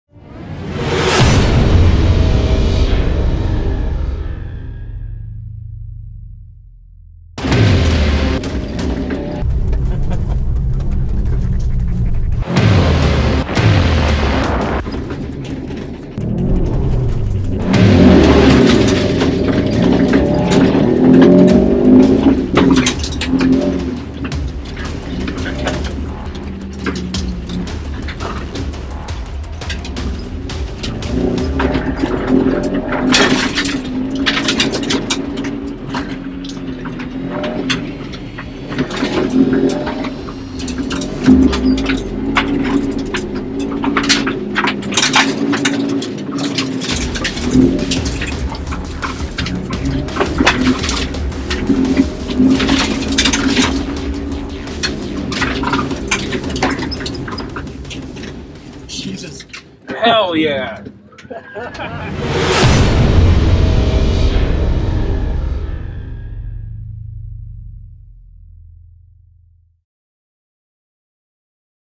COLORADO RALLYCROSS AT PPIR AUG 2024
Taking my first win in Modified AWD at Rocky Mountain SCCA Rallycross event 6 at Pikes Peak International Raceway.